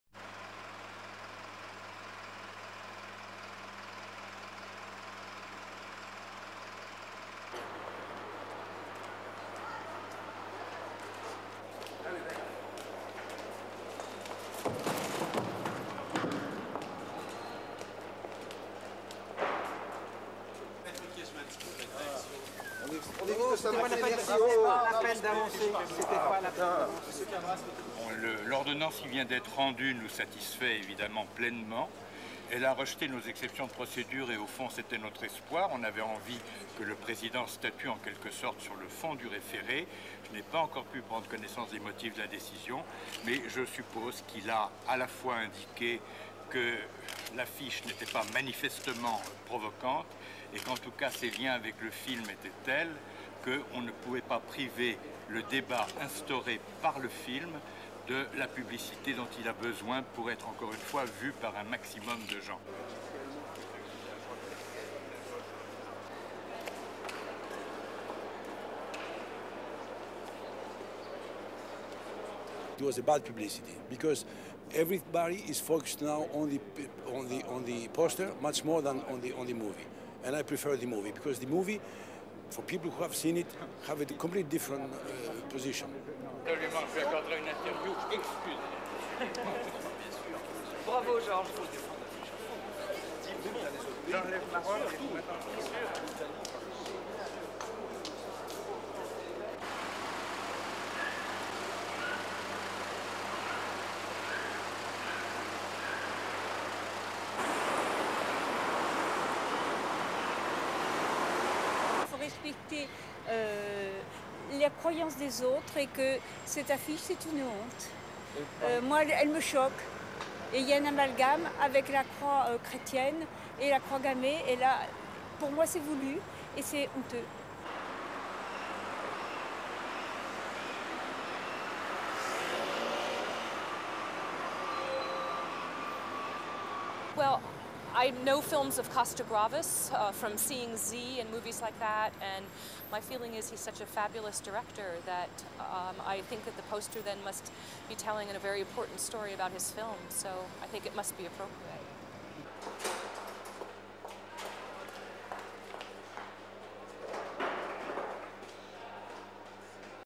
Controversial movie poster blends cross with swastika
1. Various of poster for controversial film 'Amen'
2. Exterior of Palais de Justice law courts
3. Lawyer representing director Constantin Costas-Gravas Georges Kiejman leaving court room
4. SOUNDBITE (French) Georges Kiejman, lawyer for Constantin Costas-Gravas:
"The decision that was just given completely satisfies me. It rejects the 'exception of procedure' move against us and that was what we were hoping for. We wanted the presiding judge to decide on the core of the issue. I have not had time to look at the judge's detailed explanation of the decision. But I imagine he indicated the poster wasn't provocative and also that its links to the film were such that the debate provoked by the film should not be restricted by removing publicity which encourages the largest possible number of people to see the film."
5. Various of press in halls of Palais de Justice
6. SOUNDBITE (English) Constantin Costa-Gavras, Film Director:
"It was a bad publicity (poster) because everybody is focused now on the poster much more than on the movie. And I prefer the movie because the movie, for people who have seen it, has a completely different position."
7. Various of 'Amen' posters
8. SOUNDBITE (French) Vox pop:
"One should respect people's belief so this poster is shameful. It shocks me and there is an amalgamation of Christian cross and the swastika and it's vulgar and outrageous."
9. Poster
10. SOUNDBITE (English) Vox pop:
"Well I know films of Costa Gravas' from seeing Zse and movies like that. My feeling is he is such a fabulous director that I think that the poster must be telling the very important story in his film so it must be appropriate."
11. More of poster
STORYLINE:
A French court on Thursday turned down a Catholic group's request to ban a controversial film poster that blends a cross with a Nazi swastika, ruling that the images fit with the story line of the film.
Designed by Oliviero Toscani, best known for his provocative Benetton fashion ads, the publicity poster for the upcoming film "Amen" has stirred vocal protest from Jewish and Christian religious leaders in France.
A Catholic group known as AGRIF, the general alliance against racism and for the respect of French integrity, went to court on Tuesday to argue that the poster constituted a "manifest and illicit disturbance toward Christians" and should be banned.
Lawyers for the defense argued that the poster is intended to convey the film's assertion that the Vatican bears partial responsibility for the deaths of millions of Jews during the Holocaust by having remained silent.
The Paris court ruled on Thursday that "the poster is in perfect accord with both the work it announces and the contemporary thought of the French episcopate."
The judge noted that France's Catholic Church itself said in 1997 that its silence during World War II was a mistake.
The French-produced, English-language film by Greek-born Costa-Gavras opens in France on Wednesday.
It is based on the story of an SS officer who struggled in vain to convince Protestant and Catholic leaders in Germany and the pope to condemn the Holocaust.